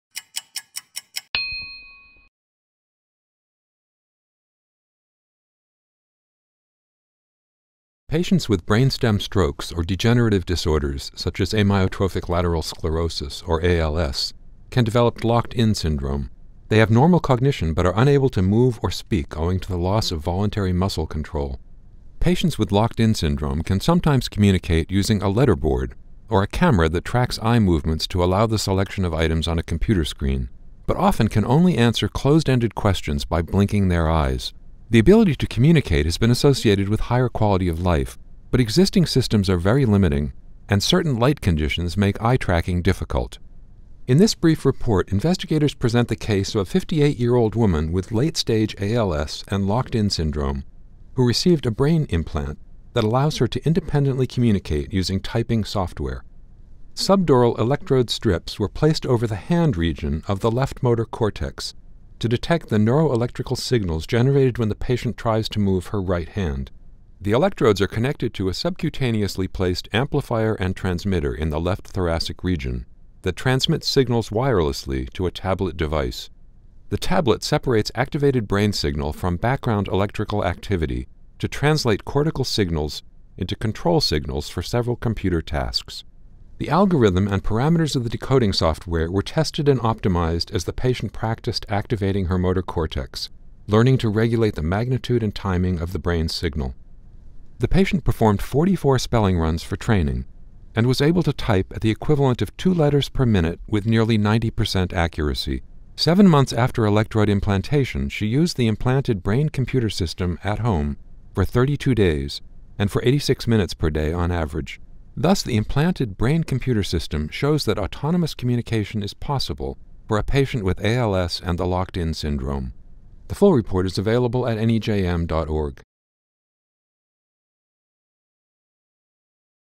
Implanted Brain–Computer Interface in a Locked-In Patient
Patients with brainstem strokes or degenerative disorders such as ALS may have normal cognition without the ability to move or speak. Could a brain–computer interface enable autonomous communication? New research findings are summarized in a short video.